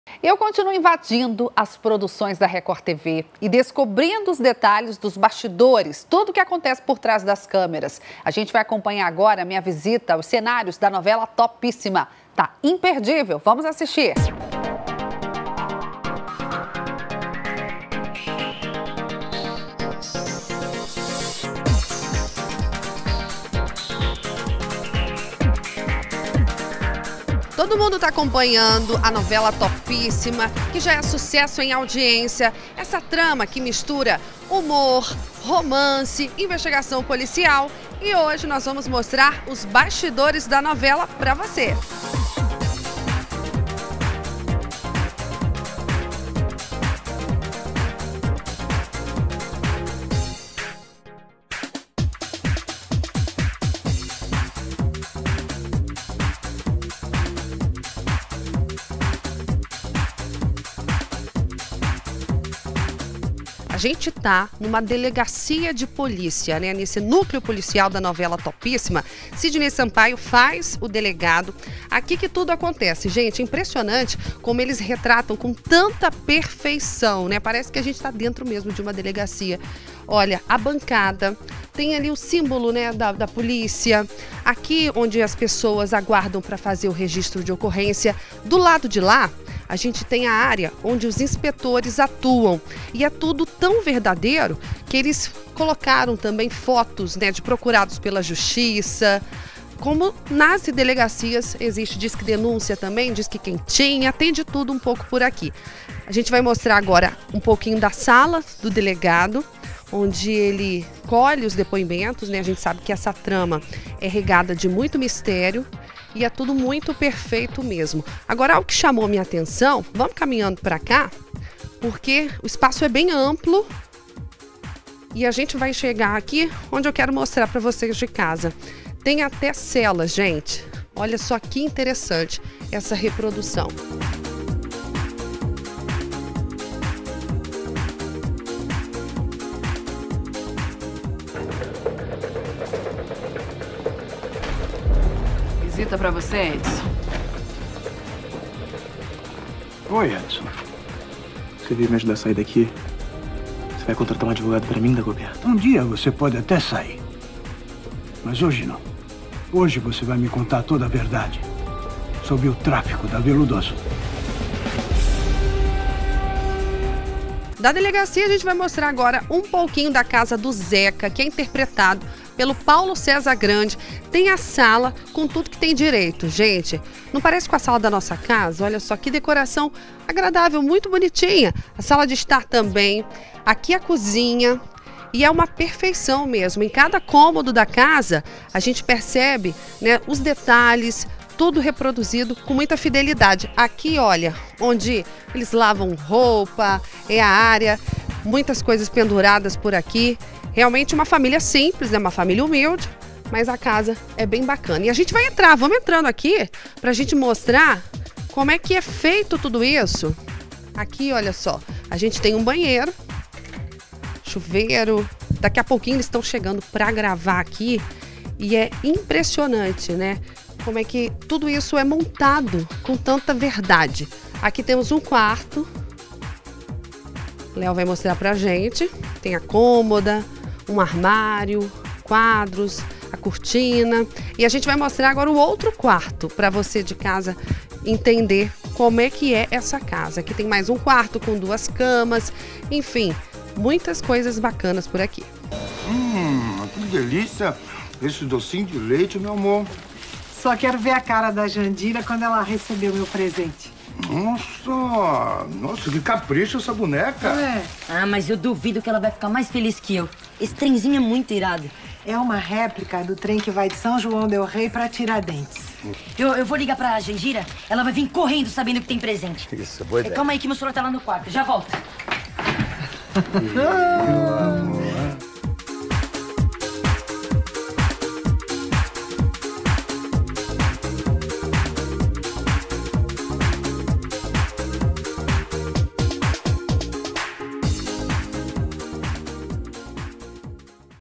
NOS BASTIDORES DA TOPÍSSIMA - Parte 1
A GENTE VAI ACOMPANHAR AGORA MINHA VISITA AOS CENÁRIOS DA NOVELA TOPÍSSIMA! TÁ IMPERDÍVEL!